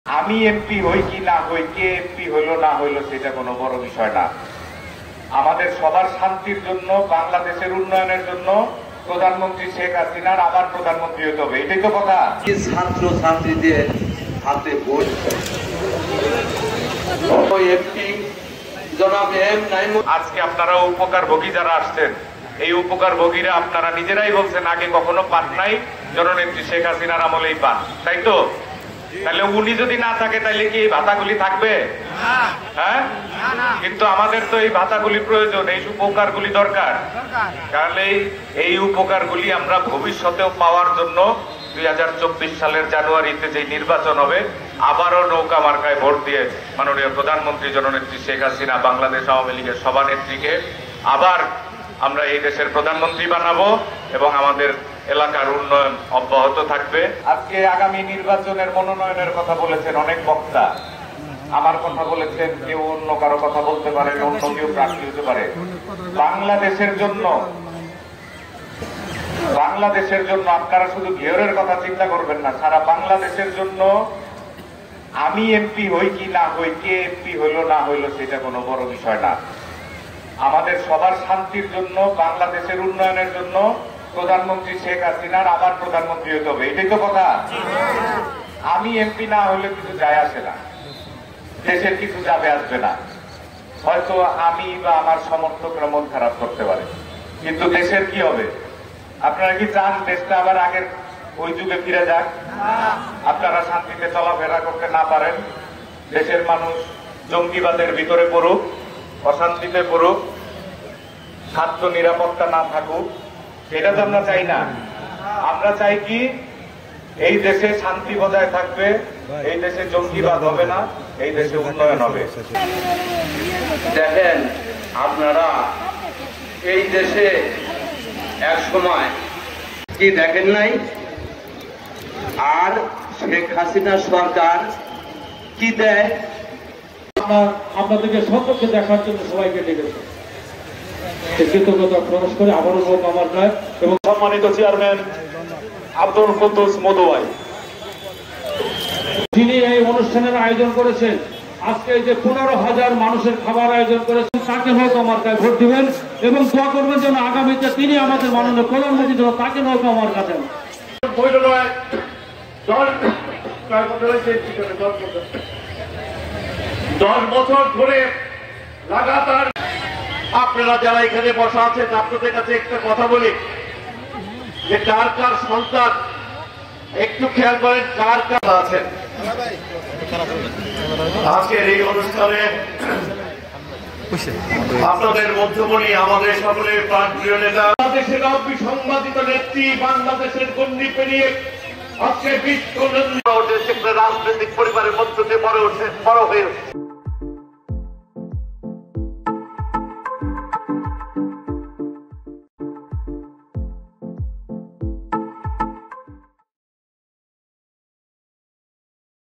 ‘আমি এমপি হই বা না হই, শেখ হাসিনাকে আবার প্রধানমন্ত্রী হইতে হবে’ | Durjoy MP | Manikganj | Samakal
‘আমি এমপি হই বা না হই, শেখ হাসিনাকে আবার প্রধানমন্ত্রী হইতে হবে’ | A.M. Naimur Rahman Durjoy MP | Manikganj

মানিকগঞ্জ-১ আসনের সংসদ সদস্য ও বাংলাদেশ ক্রিকেট বোর্ডের (বিসিবি) পরিচালক এ এম নাইমুর রহমান দুর্জয় বলেছেন, ‘আমি এমপি হই বা না হই, দেশের শান্তির জন্য, খাদ্যের নিরাপত্তার জন্য শেখ হাসিনাকে আবার প্রধানমন্ত্রী হইতে হবে। ২০২৪ সালের আবারও নৌকাকে জয়যুক্ত করতে হবে.